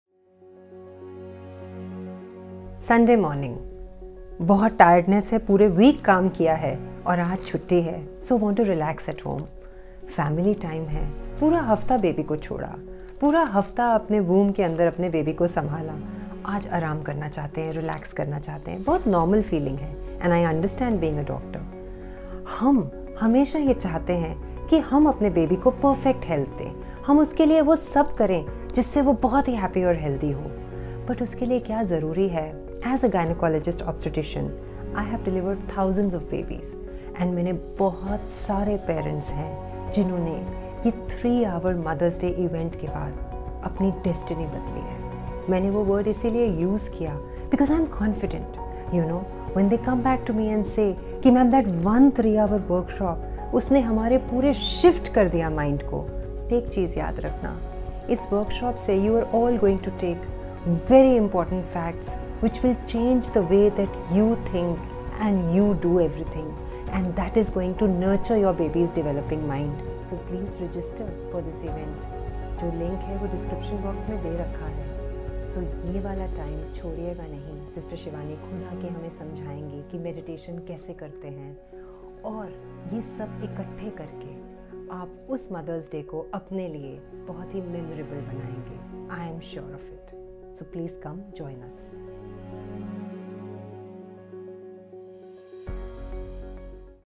Creating your Child's Personality- The Time is Now Register Now !
Stress, Anger, Fear and Worry in couples have a direct effect on the mind and lifelong health of the child. We explore scientific & spiritual principles of Garbh Sanskar before conception , during pregnancy and after the child is born. Let us all join together and celebrate Mother’s Day to create a healthy future generation.

The program is for parents planning a pregnancy ,expecting parents and parents with kids up to 2 years.

Join Sister BK Shivani and Dr. Nitika Sobti on 8th May’22 at 10.30AM

at Riveria House , Ambience Island , NH-8 ,Behind The Leela Hotel , Ambience,Gurugram.

Purchase the Virtue Baby Kit and Guided Meditation set for Happy, Healthy and Peaceful Pregnancy, childbirth and postpartum phase.

You can visit her in Cloudnine Hospital Sector-47 Gurugram
To know more: